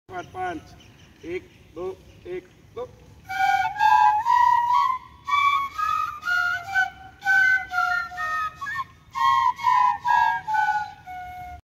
వేణువు శిక్షణ
రాష్ట్రీయ స్వయంసేవక్ సంఘ్
మదనపల్లి నగరం
ఘోష్ వర్గ (వేణువు శిక్షణ)

ప్రతి *ఆదివారం*
 *ఉదయం* 6:30am to 9:00am వరకు అభ్యాసం ఉంటుంది.

స్థలం: BT COLLEGE,
          library ground.
         Opp.Bharathi college.
         madanapalli